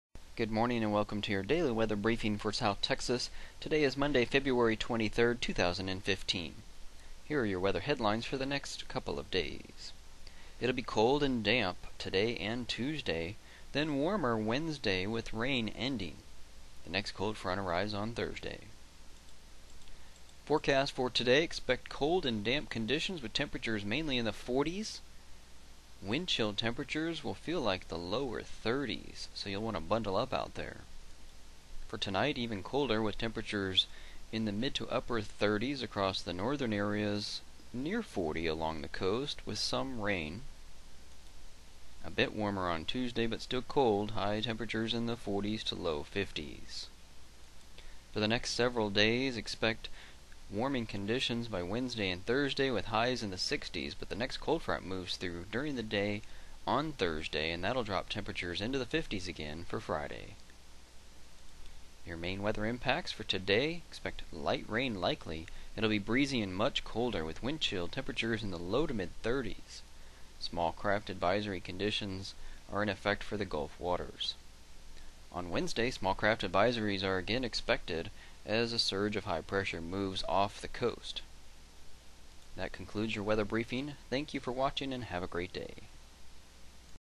90 Second Weather Briefing - Feb 23, 2015
Find out how long the cold weather will last in our latest South Texas Daily weather briefing.